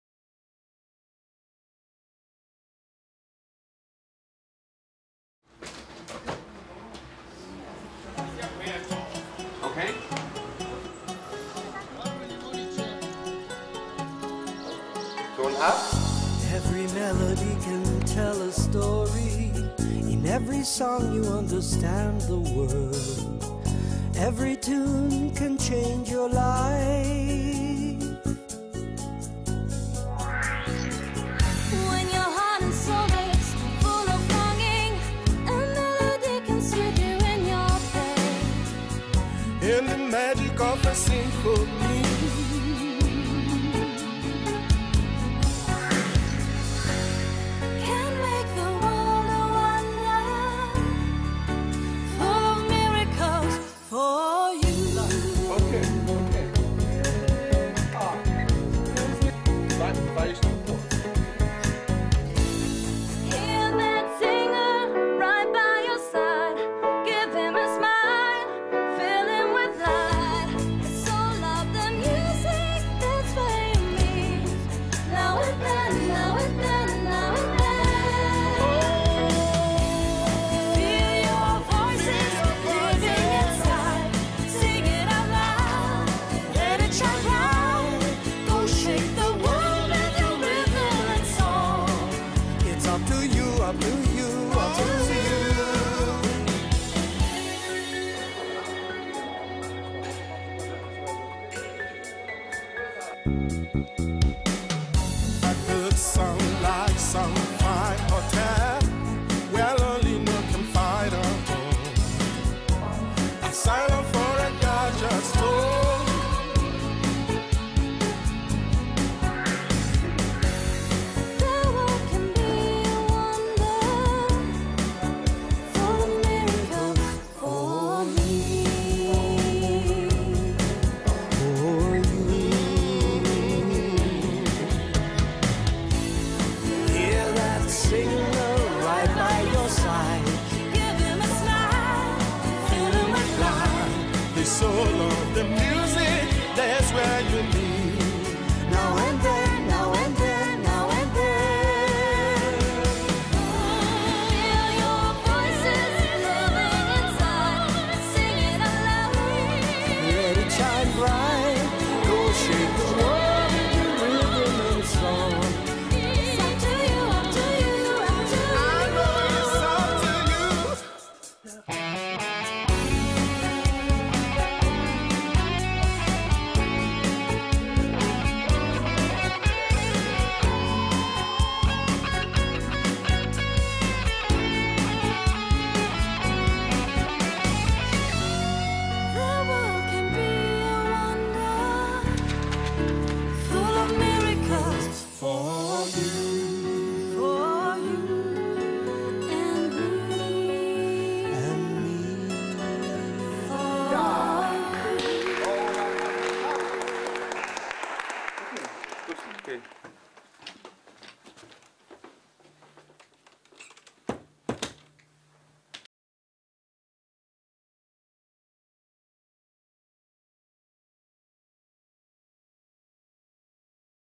Making of the TV-Spot of the 6th World Choir Games
Have a look behind the scene! The "Making of" leads you through the production of the TV-spot. Looking like being an easy job, it is actually hard work that requires instinctive feeling and the courage to go beyond one's own limits.